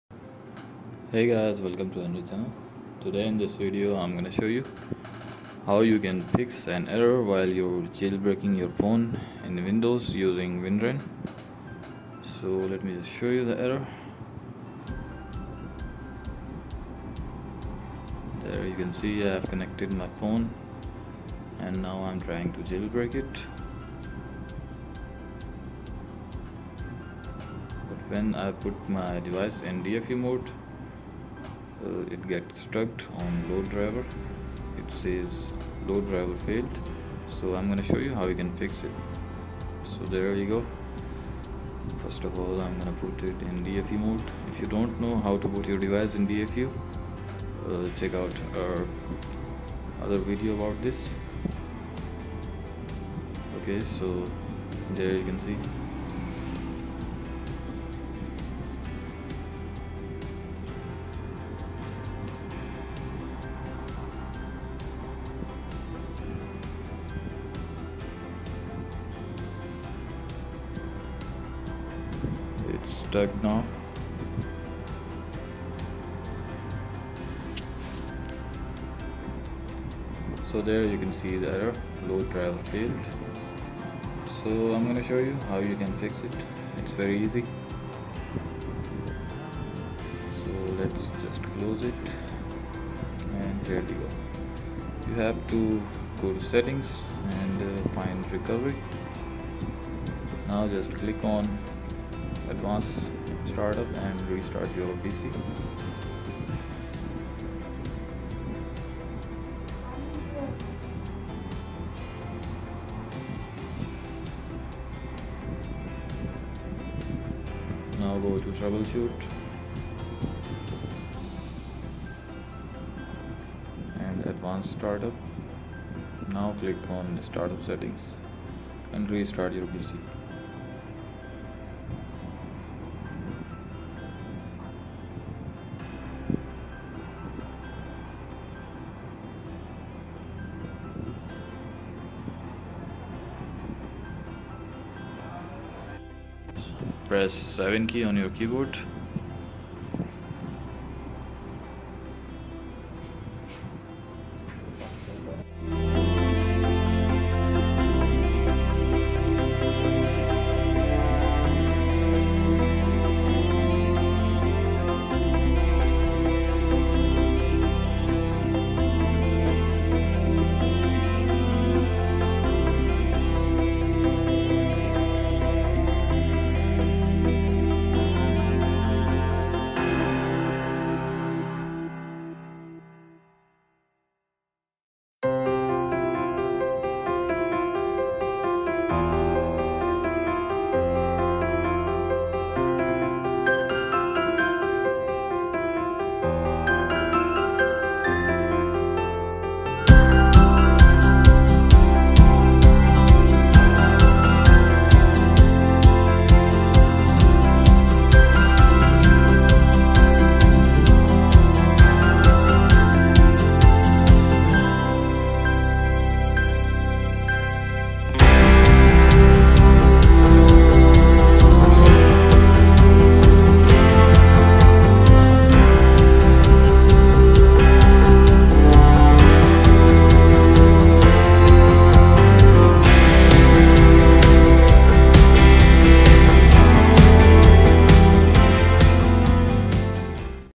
Jailbreak Error FIXED✔️: (CHECKRA1N2) (STAGE) winra1n load driver failed.
Jailbreak Error FIXED✔️: (CHECKRA1N2) (STAGE) Load Driver… ( Failed ) WinRa1n

In this video tutorial, you will learn how to fix this Jailbreak error message on your device when using Winra1n:
“(CHECKRA1N2) (STAGE) Load Driver… ( Failed )”